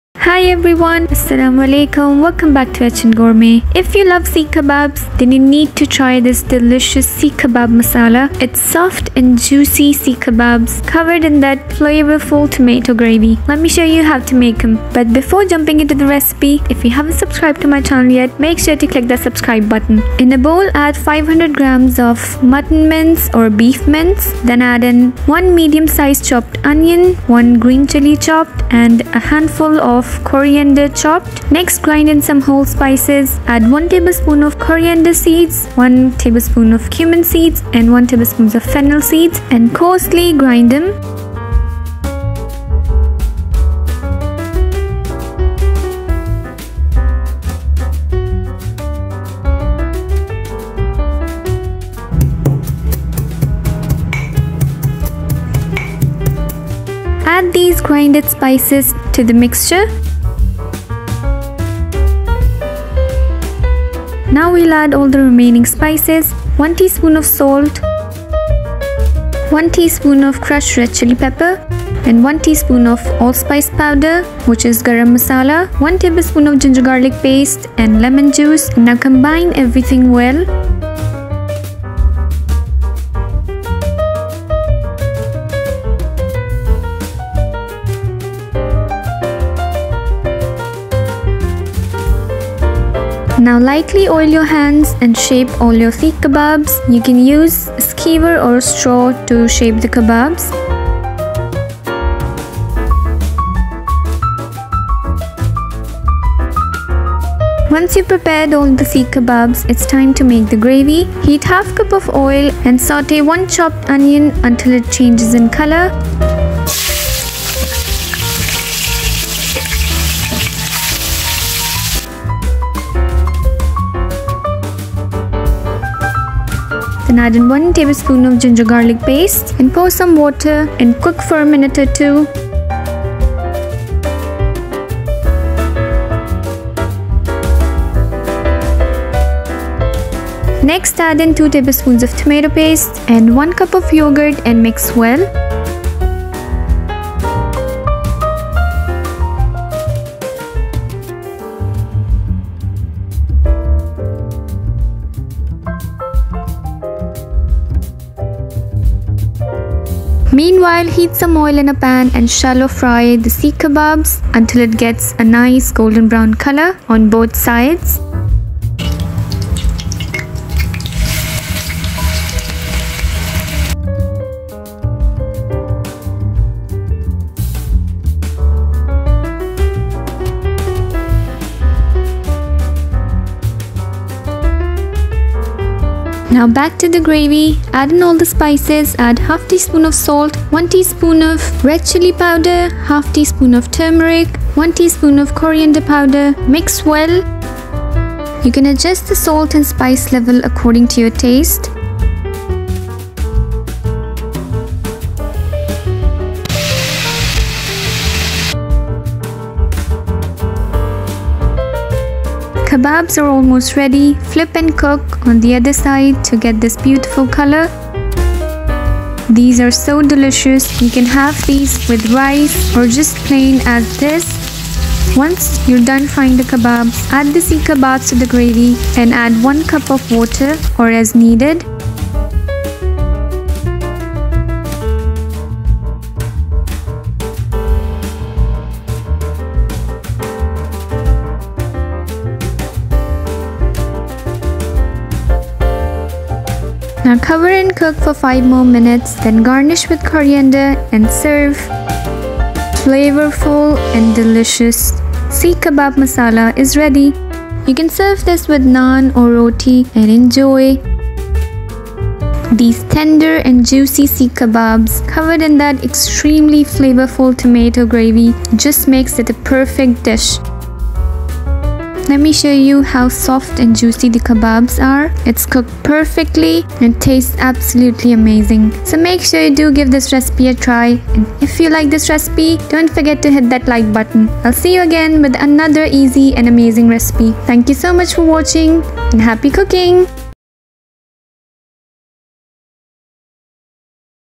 Seekh Kebab Masala Recipe | If You Love Seekh Kebab, Try this Delicious Recipe!
Seekh Kebab Masala is a delicious and juicy kebabs  recipe cooked in rice tomato based sauce.